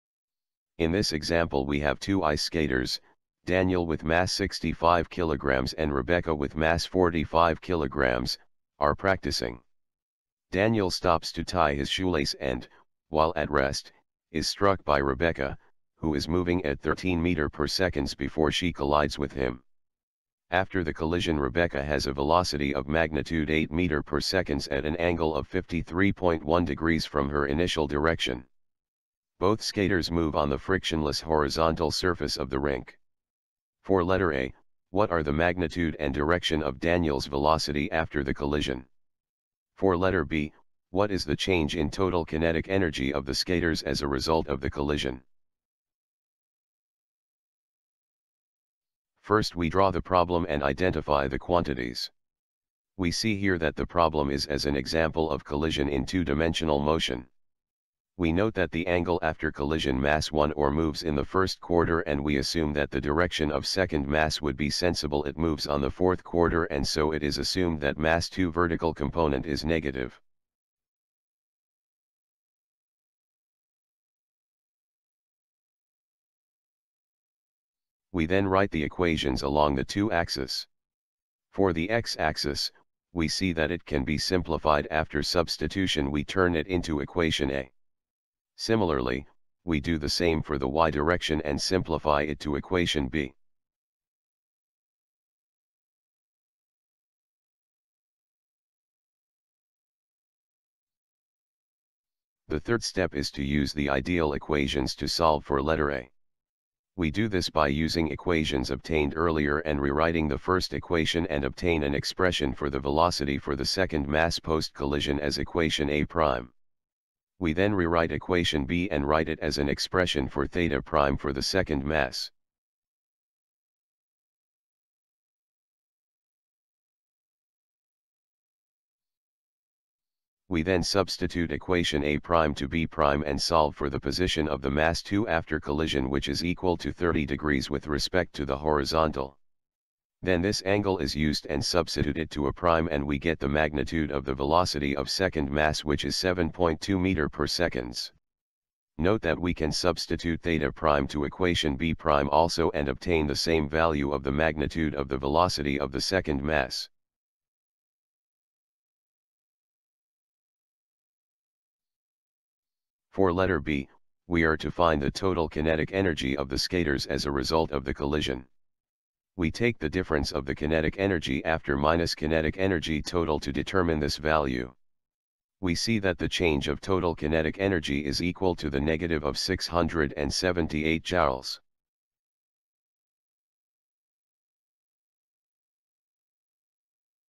Physics 01 Module 06 Example 04 Elastic Collision two dimensional
Two ice skaters, Daniel (mass 65.0 kg) and Rebecca (mass 45.0 kg), are practicing. Daniel stops to tie his shoelace and, while at rest, is struck by Rebecca, who is moving at 13.0 m/s before she collides with him. After the collision Rebecca has a velocity of magnitude 8.0 m/s at an angle of 53.10 from her initial direction. Both skaters move on the frictionless horizontal surface of the rink.
 a) What are the magnitude and direction of Daniel’s velocity after the collision?
 b) What is the change in total kinetic energy of the skaters as a result of the collision?